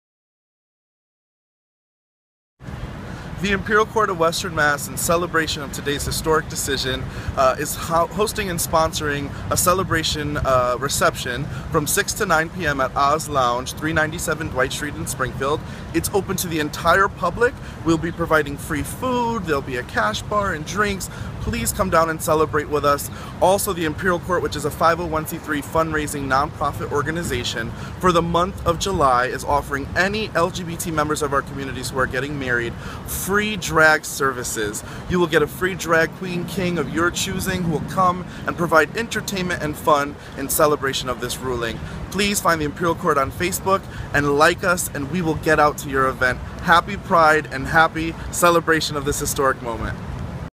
LGBT Activist Nelson Roman of Holyoke ready to celebrate gay marriage ruling at Oz Lounge party to t
The Imperial Court of Western Mass is sponsoring tonight's party in Springfield .